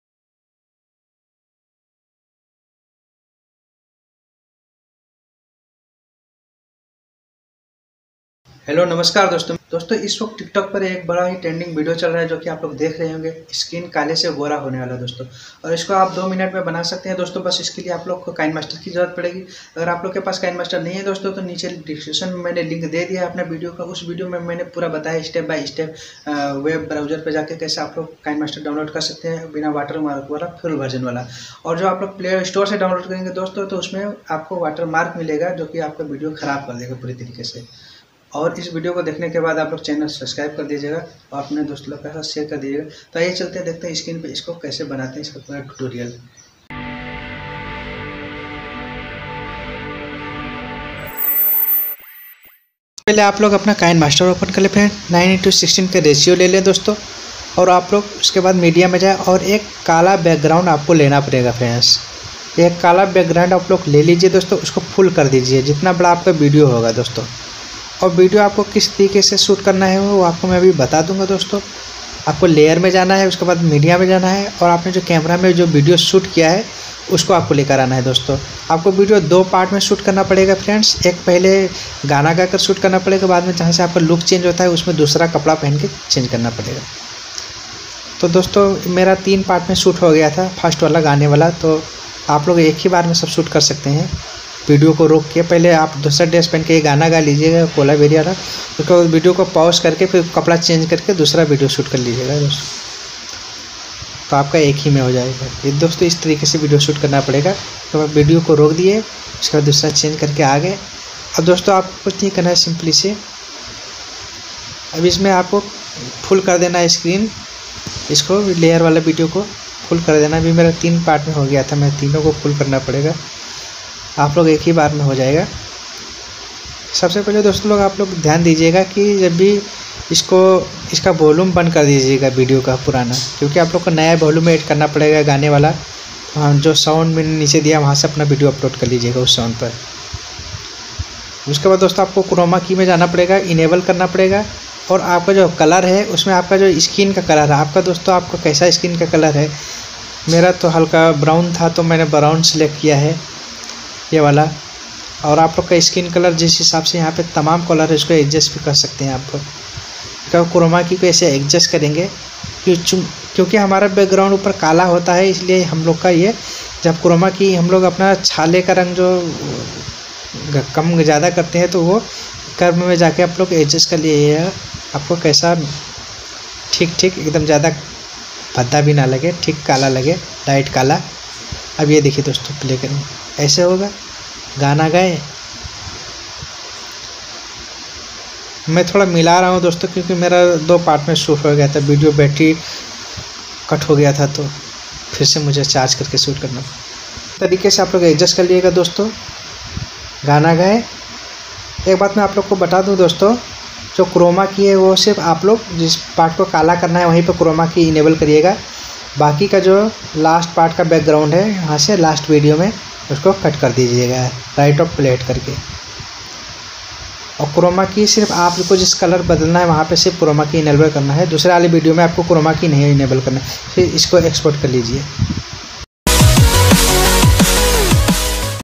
Tiktok new black and white transformation editing || tiktok new trend || kinemaster video editing
Dost aaj is video mein Ham aapko bataenge ki aap log Kale se gora hone wala transformation video tik Tok kaise bana sakte hain iska pura tutorial Maine bataya hua hai iske liye aap log ko ek app ki jarurat padegi jiska naam hai kinemaster kinemaster Kahan se download Kare full version wala niche maine description me link de diya hai aap vahan se download kar sakte hain video dekh kar.

Kinemaster without watermark full version yahan se download Karen.